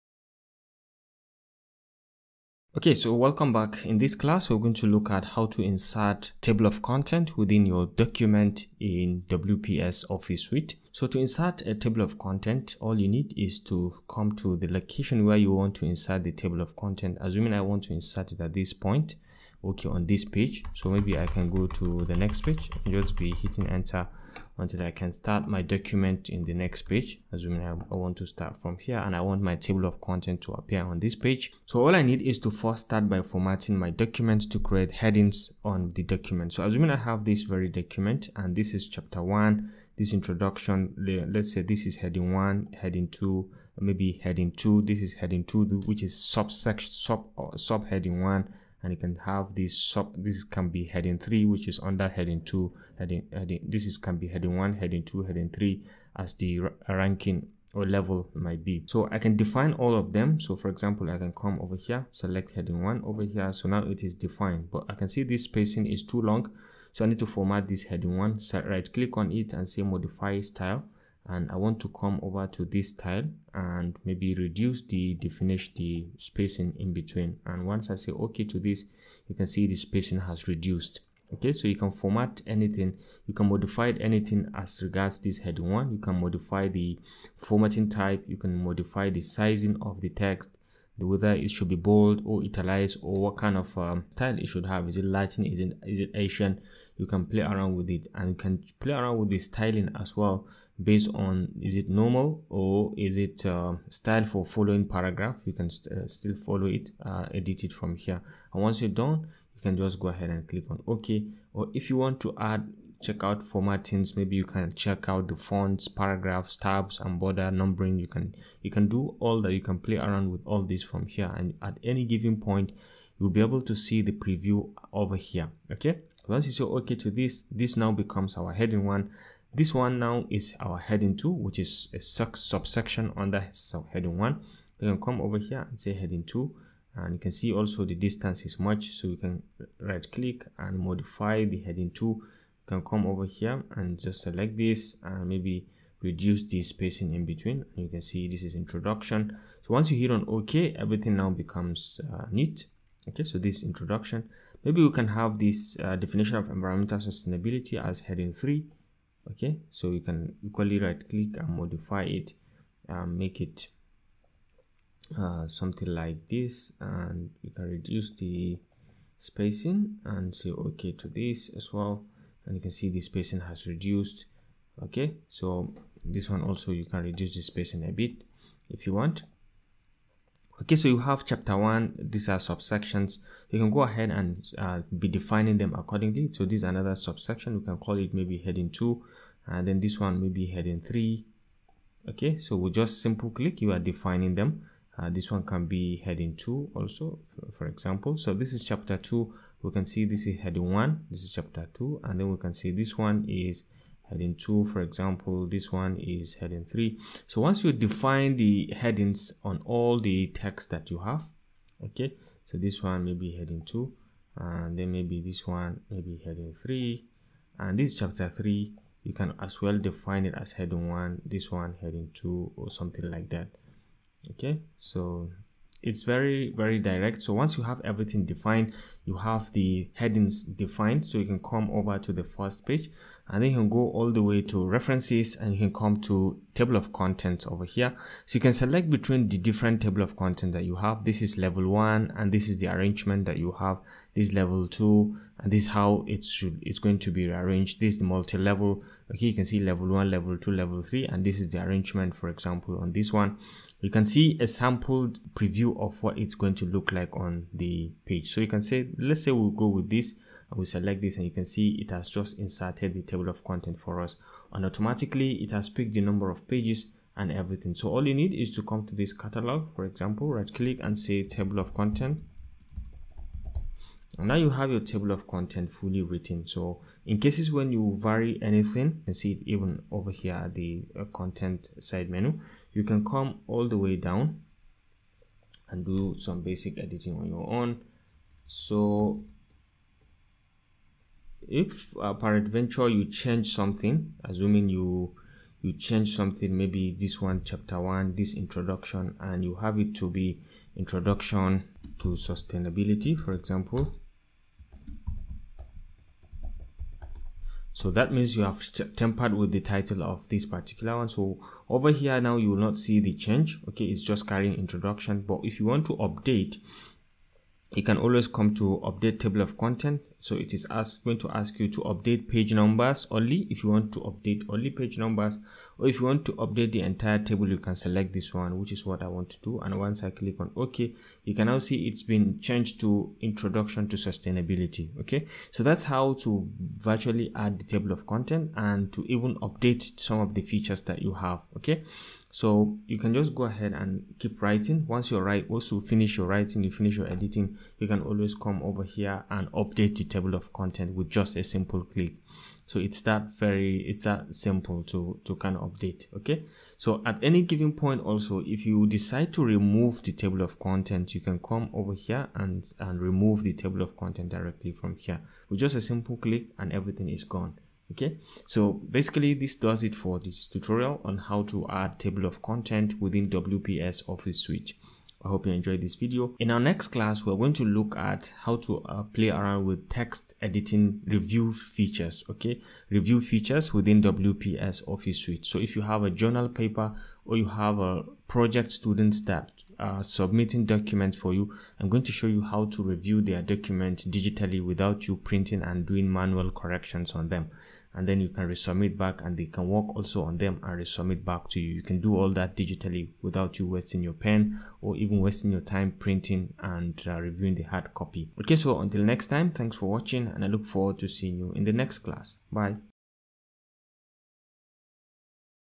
1.17 Generating Table of Content in WPS Office Suite
Learn how to effortlessly generate a table of contents in WPS Office Suite with this step-by-step tutorial. In this video, we'll guide you through the process of creating an automated table of contents that dynamically updates as you add or modify headings in your document. Discover how to apply heading styles, customize the table of contents format, and navigate through your document using the table of contents. Whether you're working on a lengthy report, thesis, or any document with multiple sections, WPS Office Suite provides the tools you need to create a professional and organized table of contents. Join us as we explore the world of generating a table of contents in WPS Office Suite and enhance the navigation and structure of your documents. Watch the video now and learn how to generate a table of contents in WPS Office Suite for improved document organization and usability.